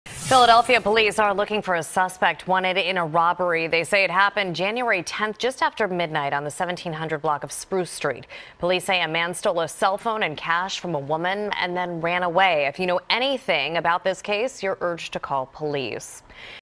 Suspect Steals Cellphone, Cash From Woman On Spruce Street
Philadelphia police are looking for a suspect wanted in a robbery.